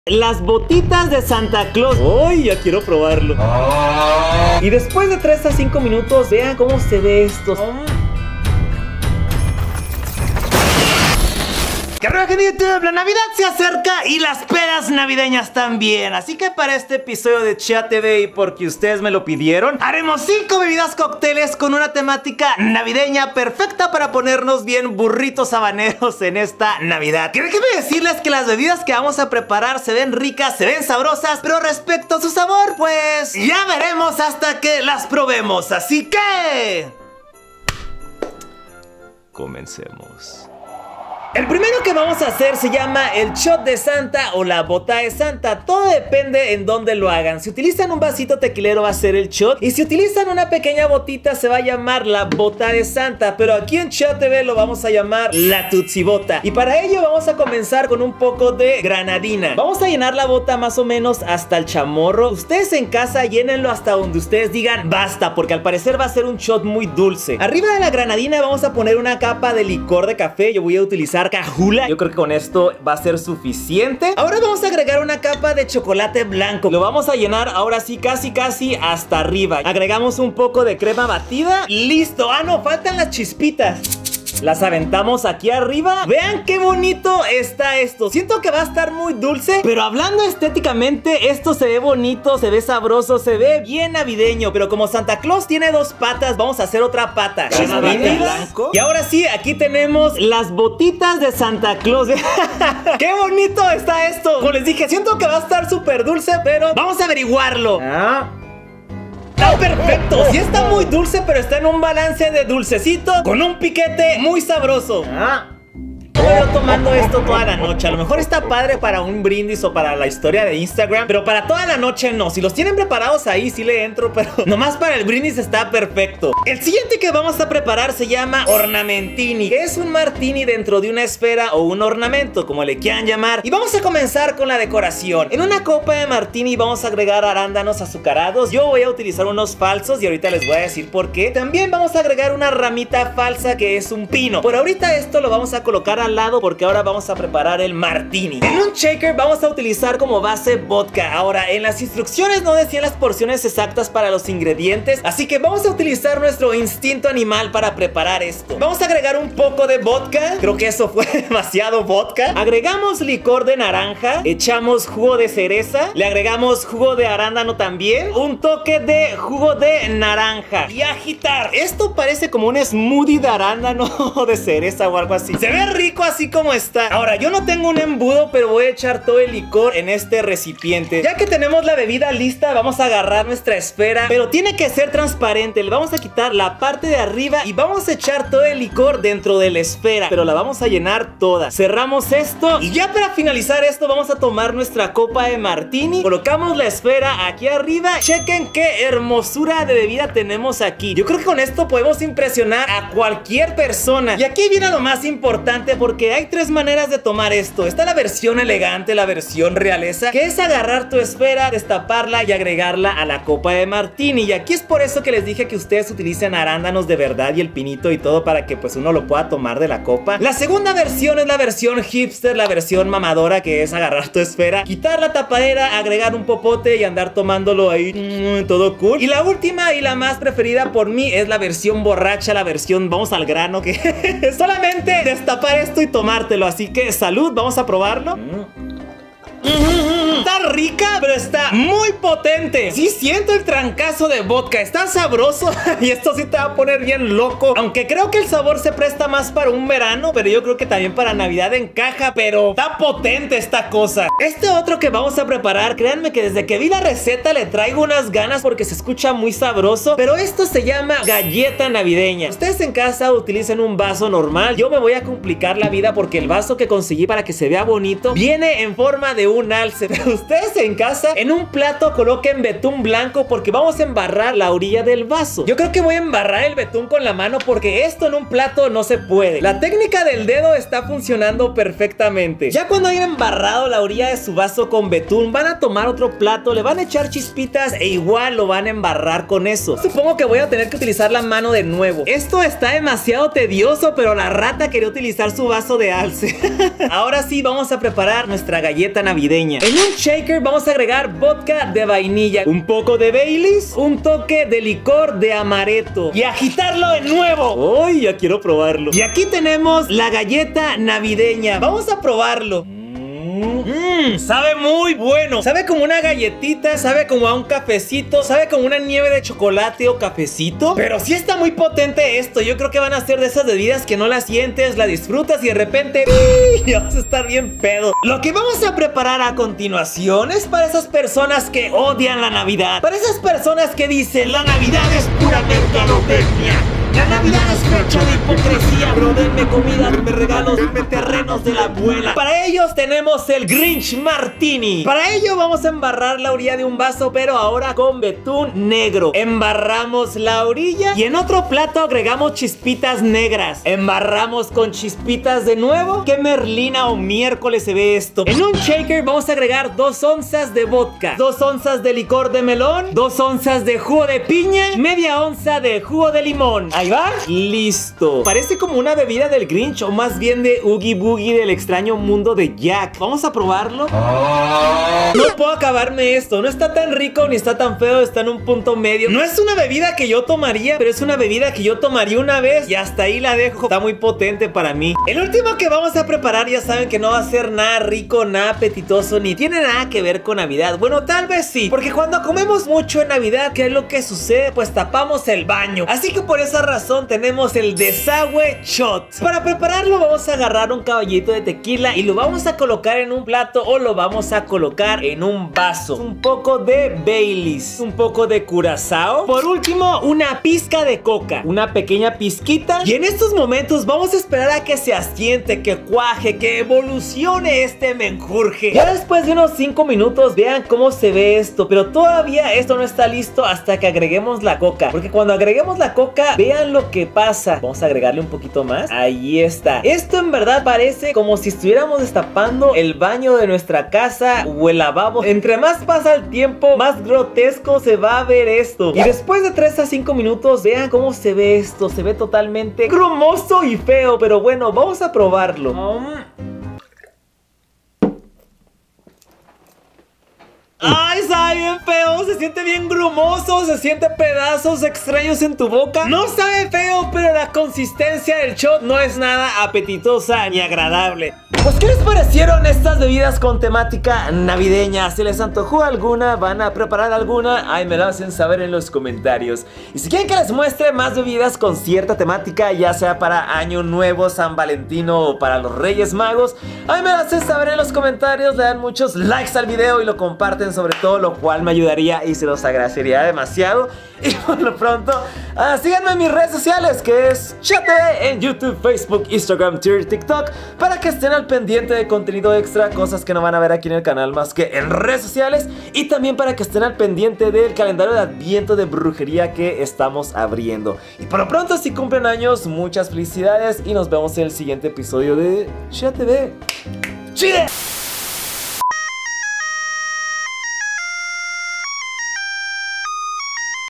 5 Tragos divertidos para Navidad!!!🎄🎅
Si no sabes que tomar en navidad, hoy te tenemos unas cuantas recetas para que te pongas bien rodolfo en estas fiestas navideñeeees...